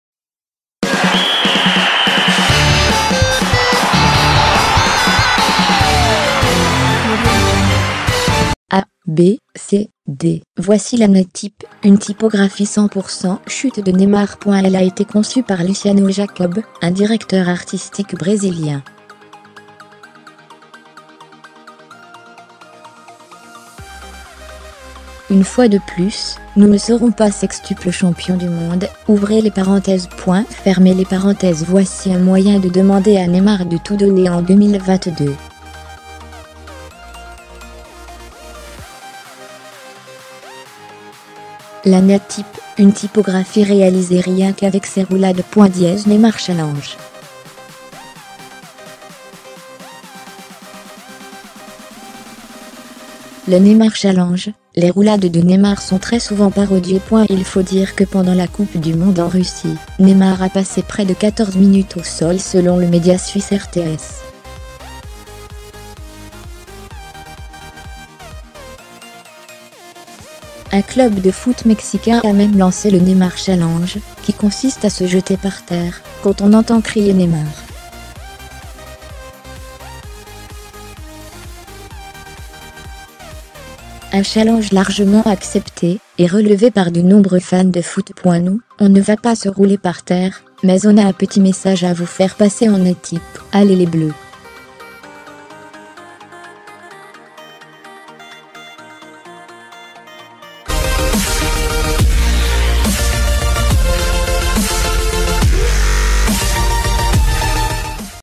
Une typographie 100% Neymar
A, B, C, D : voici la Ney Type, une typographie 100% chutes de Neymar.Elle a été conçue par Luciano Jacob, un directeur artistique brésilien."Une fois de plus, nous ne serons pas sextuples champions du monde (.)Voici un moyen de demander à Neymar de tout donner en 2022.La Ney Type, une typographie réalisée rien qu'avec ses roulades.neymarchallenge".Le "Neymar Challenge" Les roulades de Neymar sont très souvent parodiées.Il faut dire que pendant la Coupe du monde en Russie, Neymar a passé près de 14 minutes au sol selon le média suisse RTS.Un club de foot mexicain a même lancé le "Neymar Challenge", qui consiste à se jeter par terre quand on entend crier Neymar.Un challenge largement accepté et relevé par de nombreux fans de foot.Nous, on ne va pas se rouler par terre, mais on a un petit message à vous faire passer en Ney Type : "Allez les Bleus !".